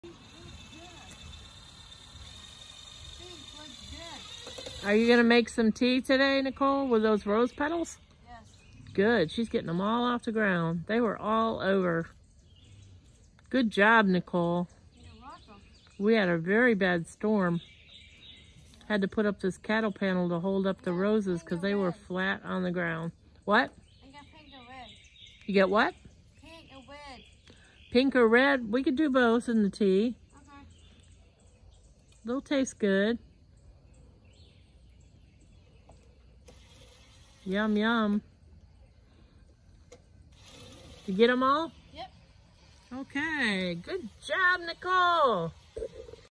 Storm roses and saving petals for tea
Hey everyone today Nicole is going to help me pick up the fallen rose pedals , we will make tea with these.